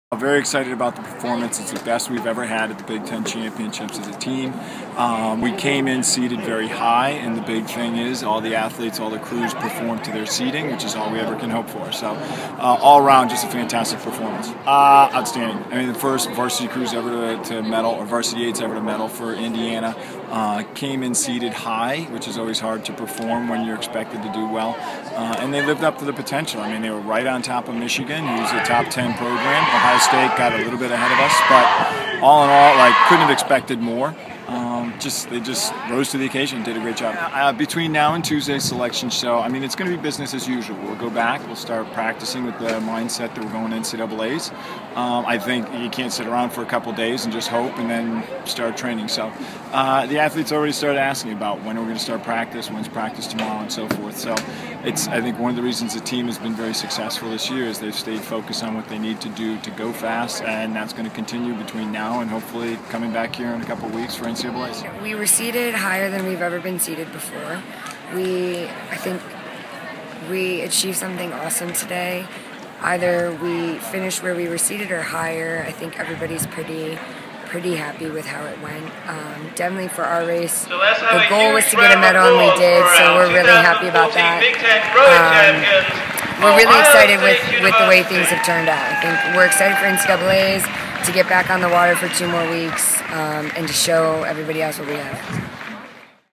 2014 Big Ten Rowing Championships Recap
Head coach Steve Peterson and junior coxswain Karly Kikkert recap the 2014 Big Ten Rowing Championships.